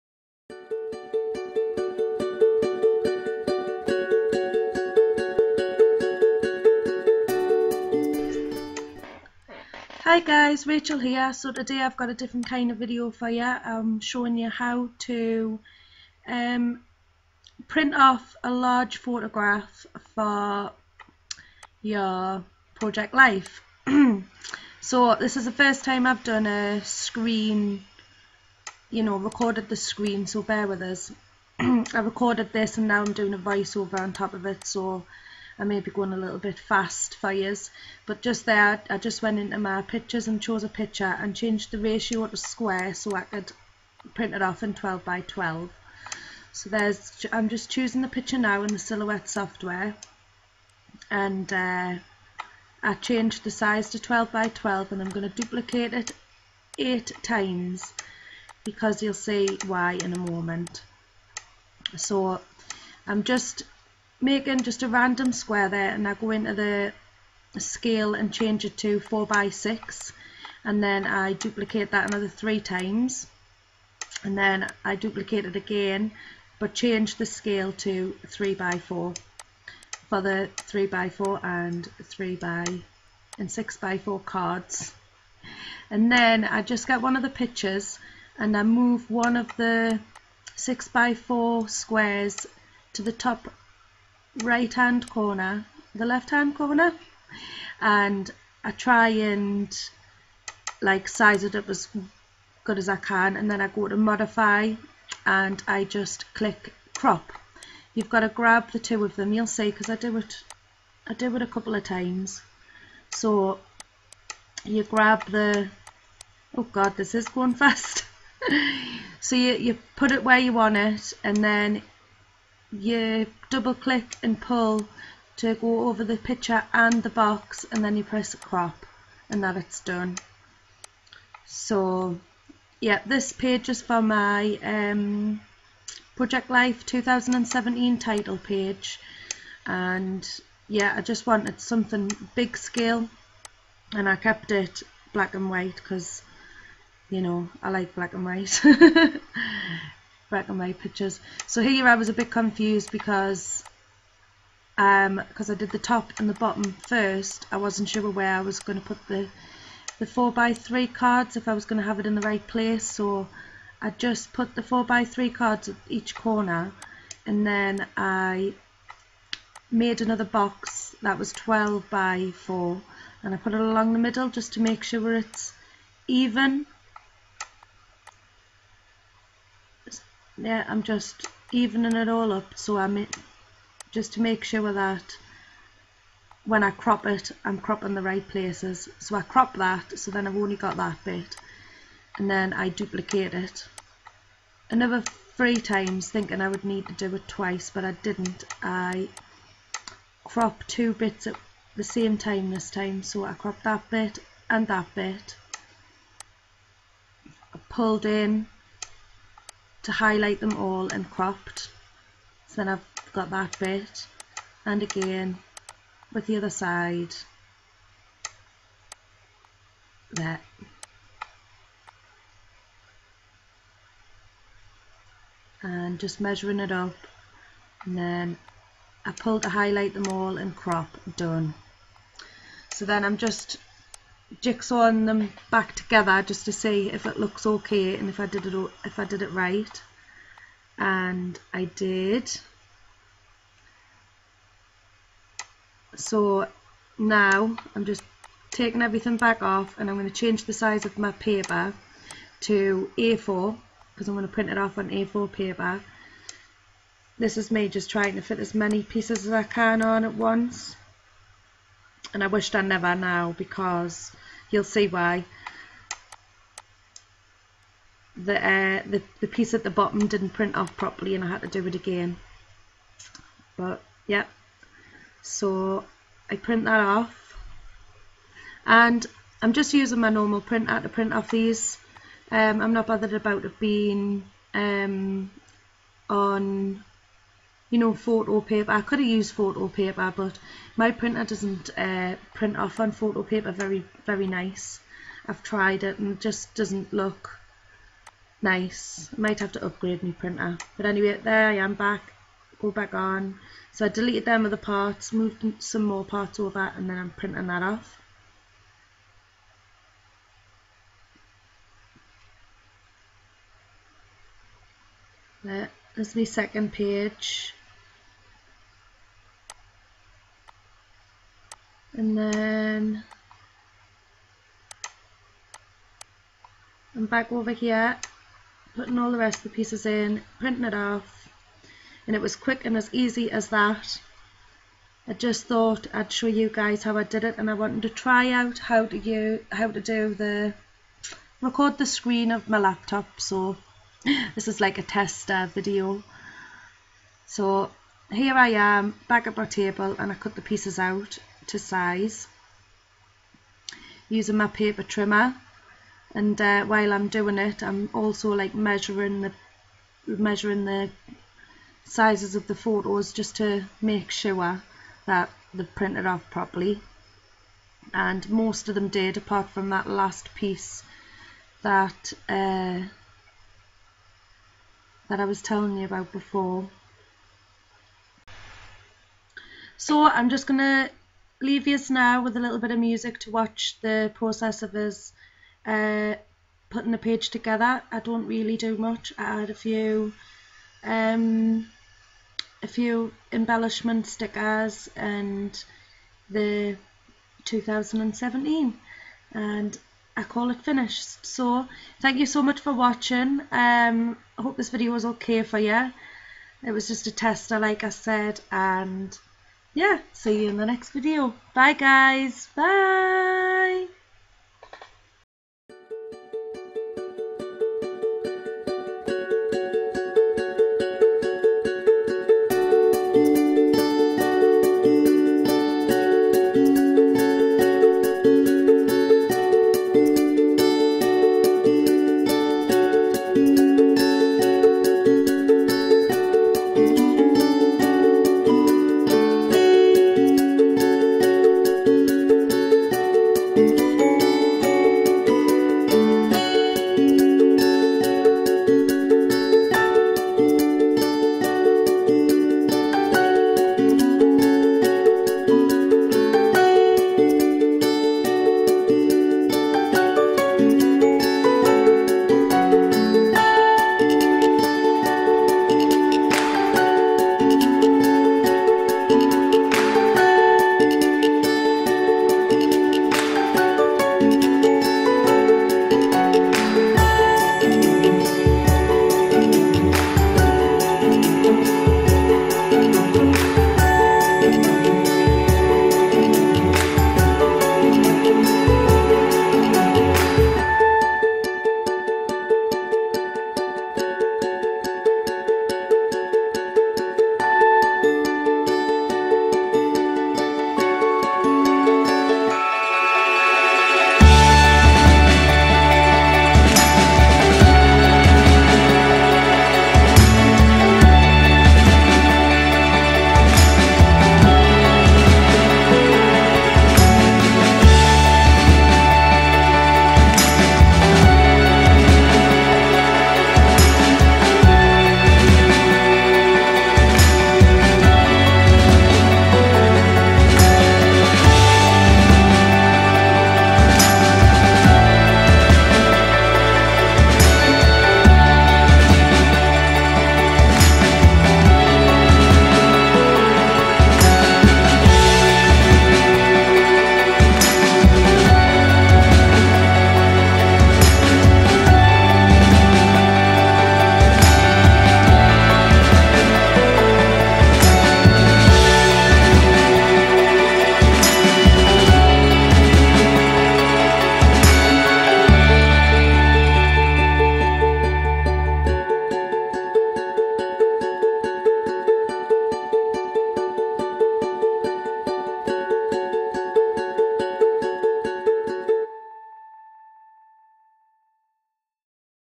Project Life 2017 - Title Page // How to enlarge a photo to fit inside a 12 x 12 PL pocket page
Hello Crafty Peeps :) I am getting back into my Project Life grove and started with my title page...check out how I enlarged a photo to fit a full 12 by 12 layout.